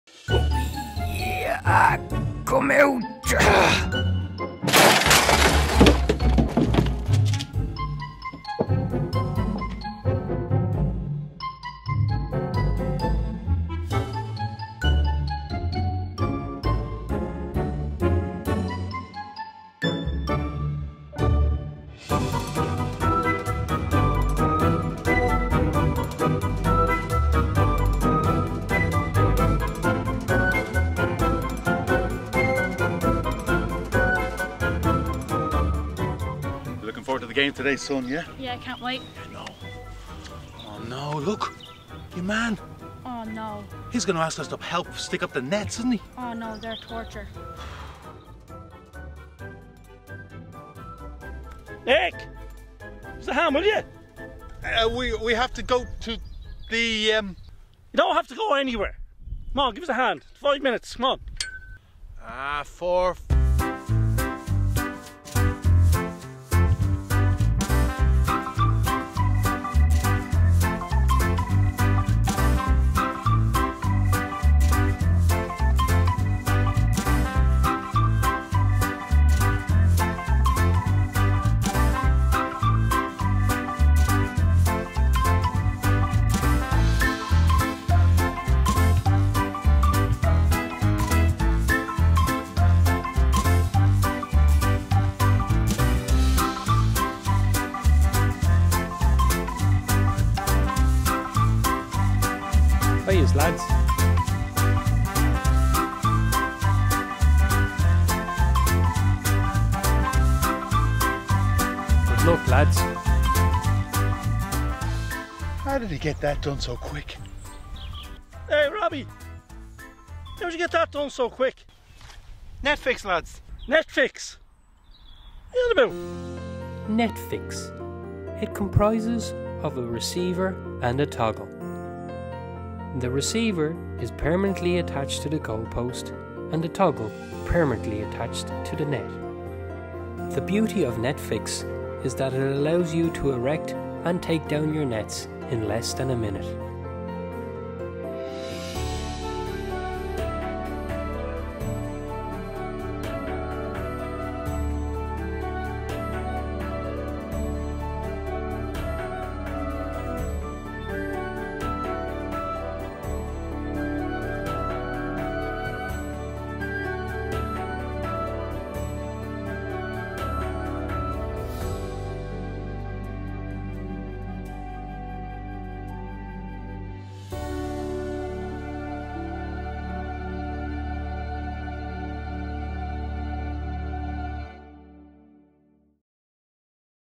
Net Fix Advert 3
Net Fix Ireland are an Irish based company that supply goal mouth products to both the GAA and Soccer associations. This video is a short promo for one of their main products, the Net Fix Receiver and Toggle.